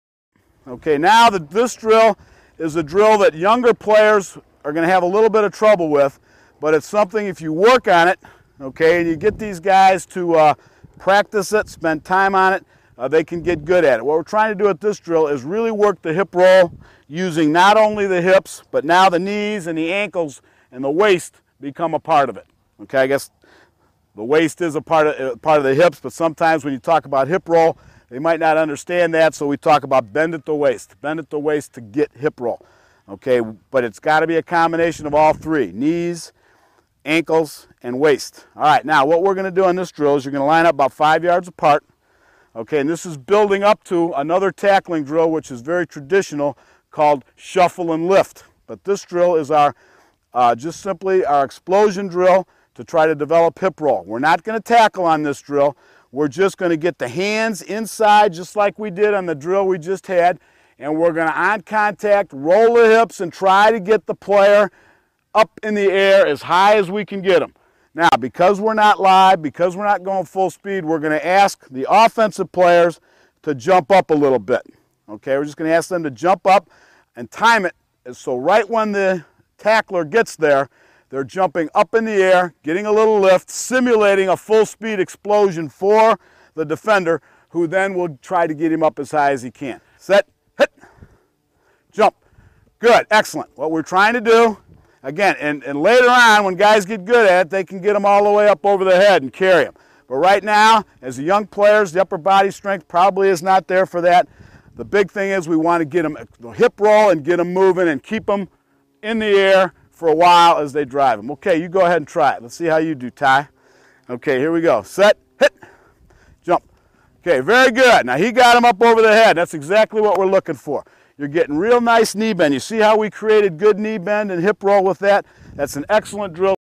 "Explosion Lift" Tackling Drill for Working the Hip Roll
Check out my blog for more FREE drills, tips and techniques!  This video focuses on the explosion of the tackle and developing the hip roll.  You are going to work on getting the hands inside, and on contact, roll the hips and try to get the player in the air as high as you can get him in the "Explosion Lift" Tackling Drill.  "Like" if you want to see more videos like this!